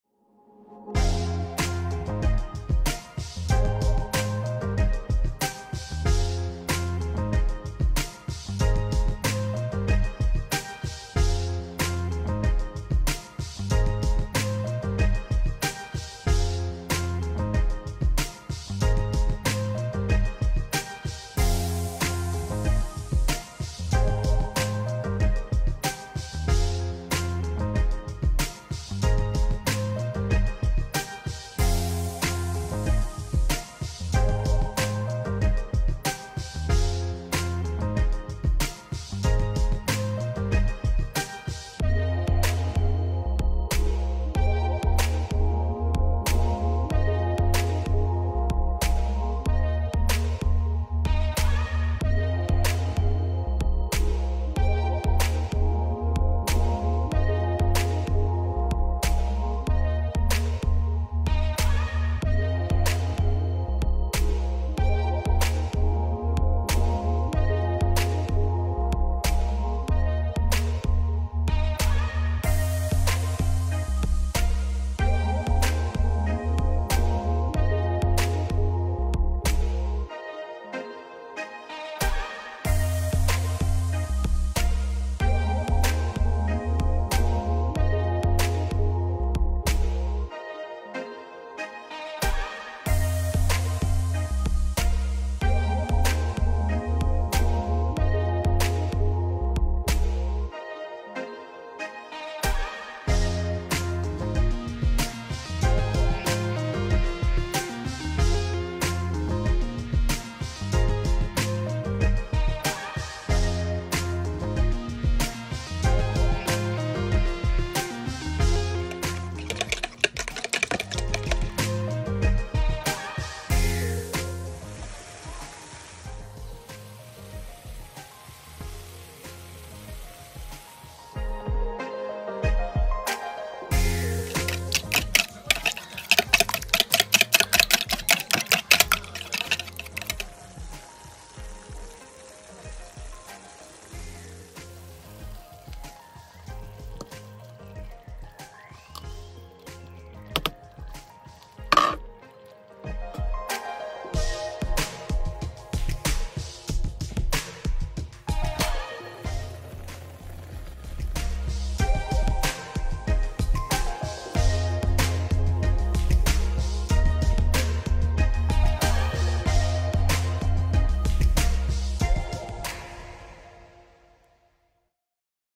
Fennel omelette
How to use fennel tops, leaves. A recipe from Turkish countryside.